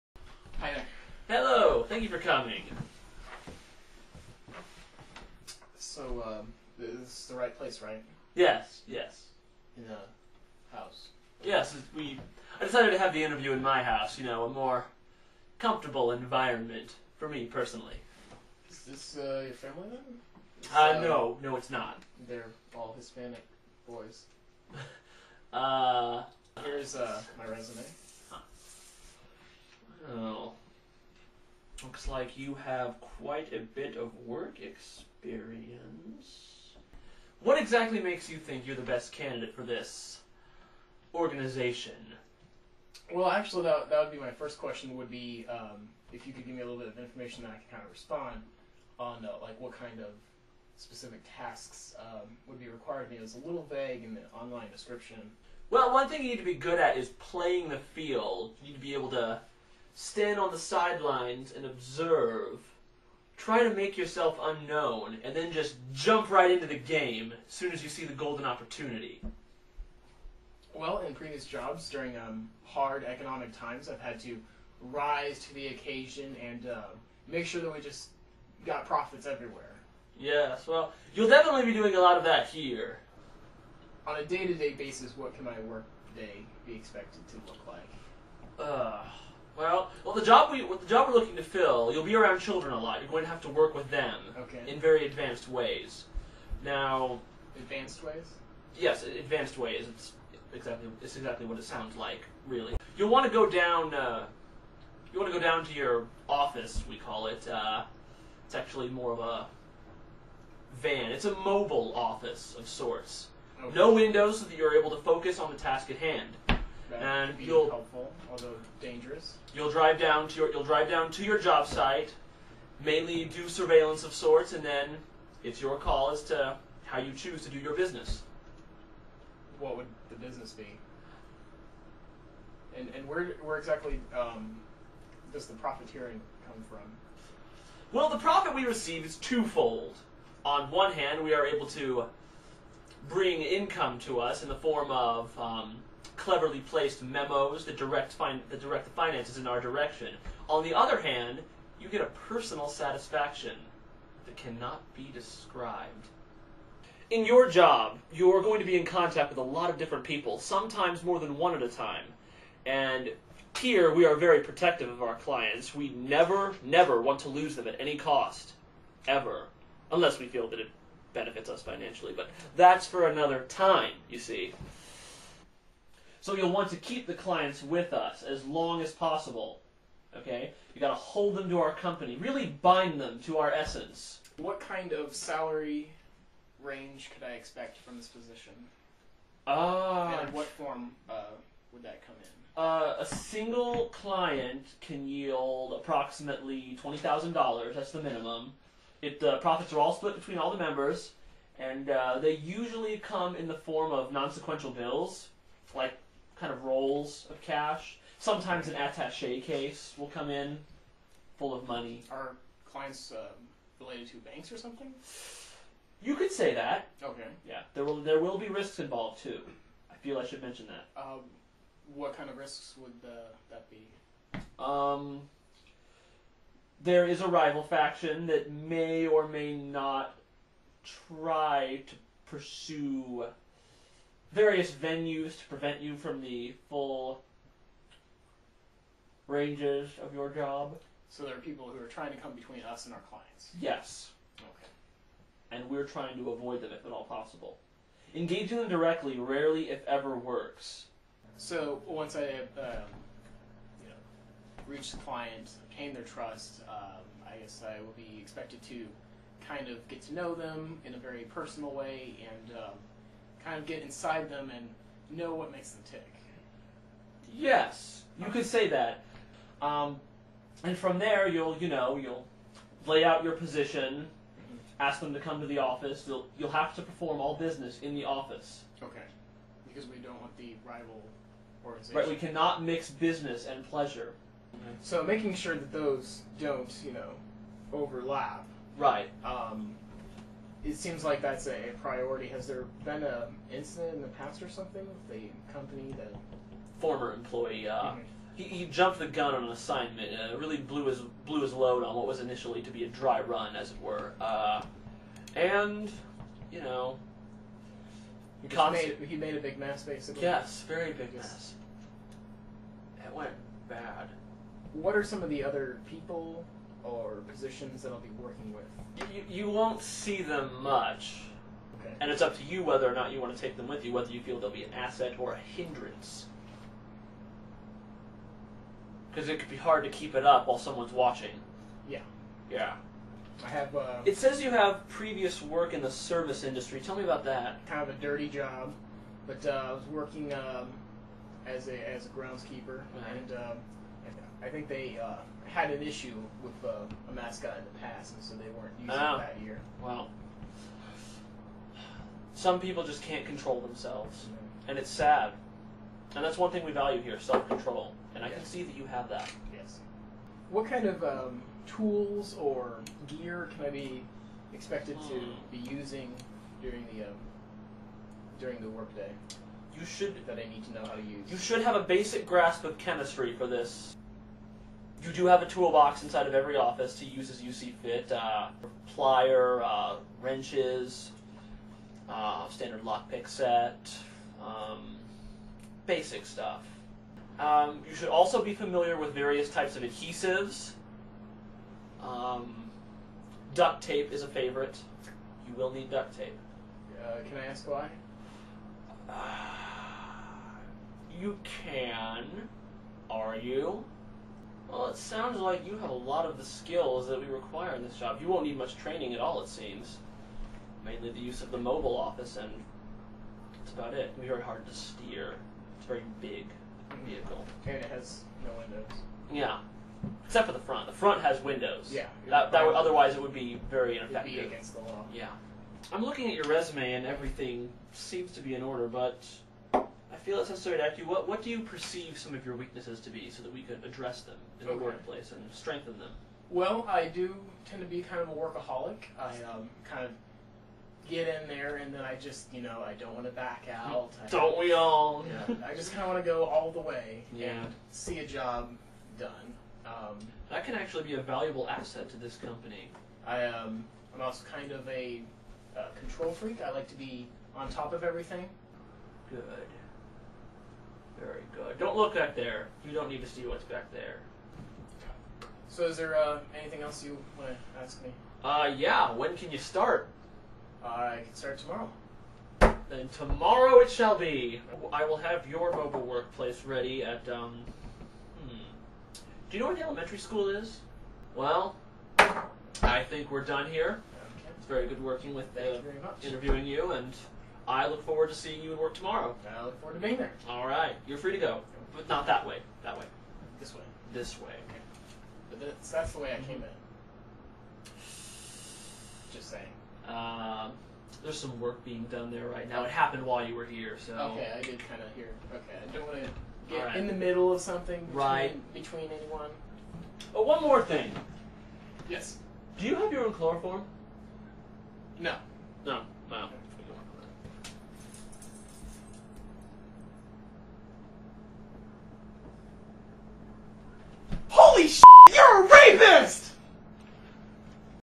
A Very Strange Job Interview
It started as an ordinary job interview that SMN found while browsing online...

I tell ya, every video I make now is a hassle. Not only did we shoot a half hour of footage, then when I was converting the clips, they wouldn't transfer with sound, and when I found a program that'd do the job, I apprently didn't save them in a high enough quality. I apologize for that last bit, which will be addressed in future videos.